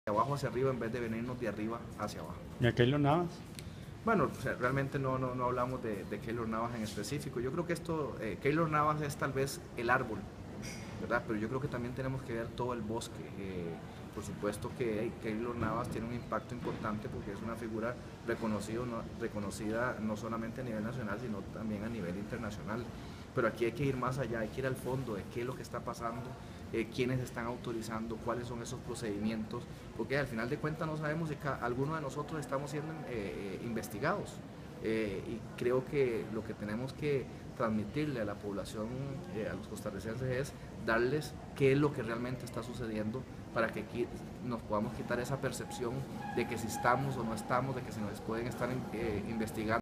Diputado PUSC, Johnny Leiva
No se habló de llamar a comparecer a Keylor Navas, pero si será el caso que permita investigar el fondo del problema